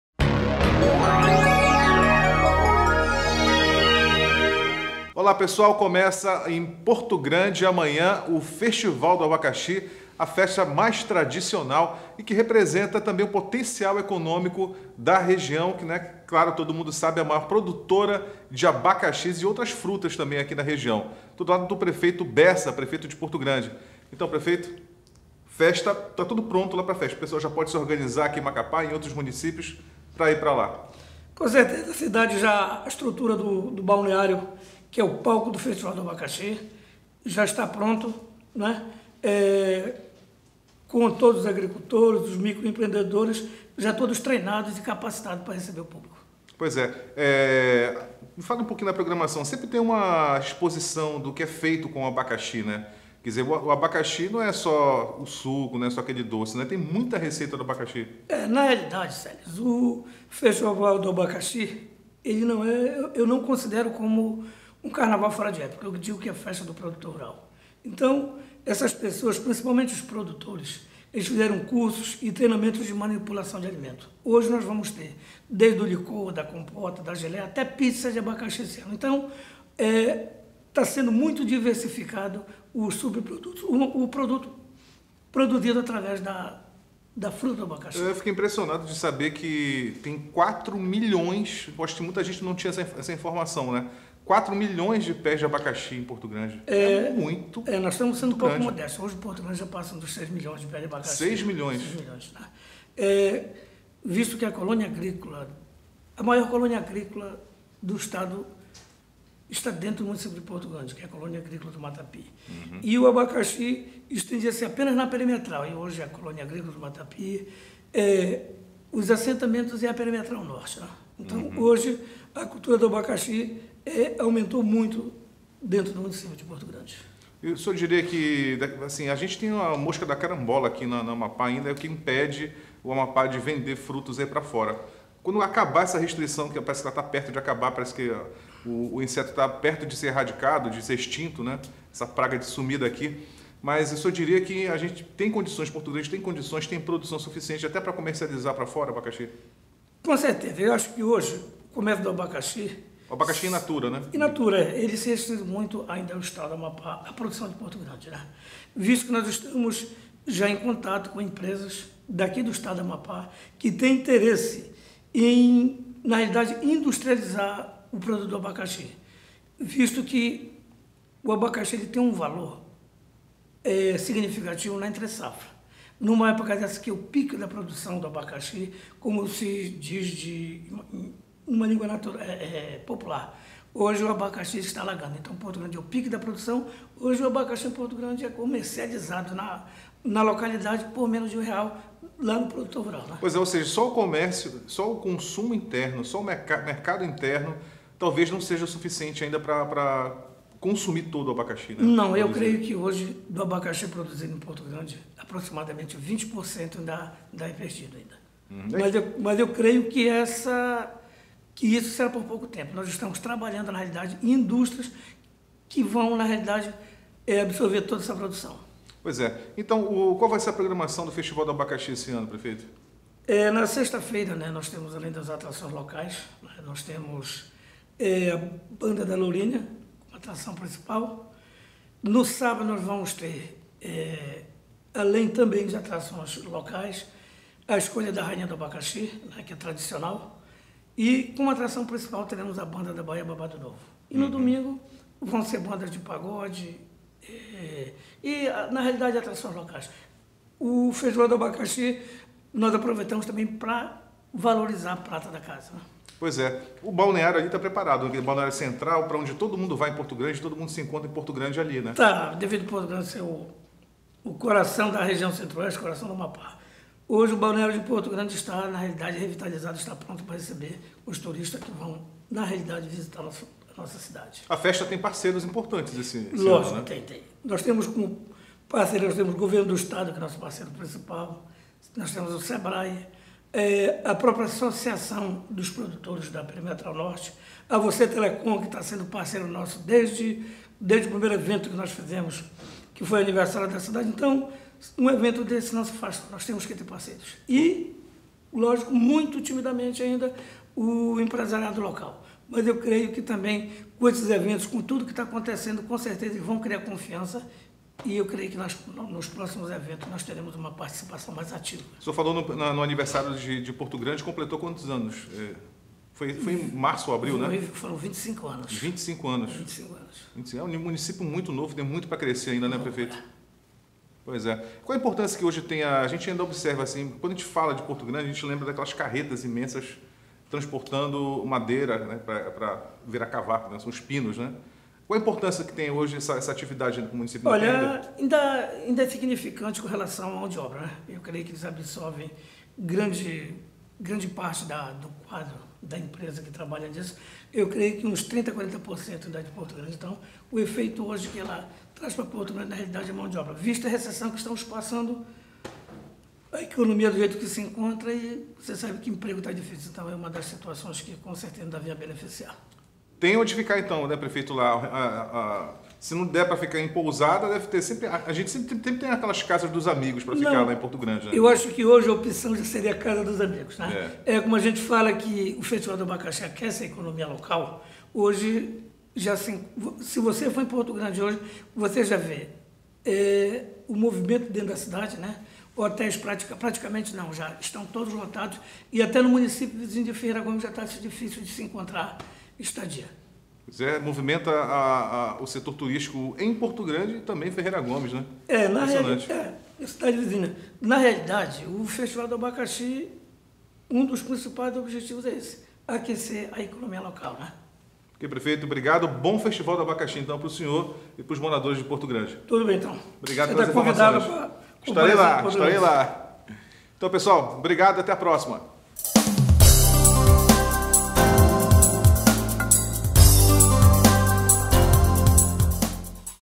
Prefeito Bessa, de Porto Grande, fala sobre o Festival do Abacaxi
Prefeito Bessa, de Porto Grande, fala sobre o Festival do Abacaxi, e revela que o município tem plantados mais de 6 milhões de pés da fruta que virou símbolo da economia na região. Empresas já estão interessadas em industrializar o fruto. Saiba mais em SELESNAFES.COM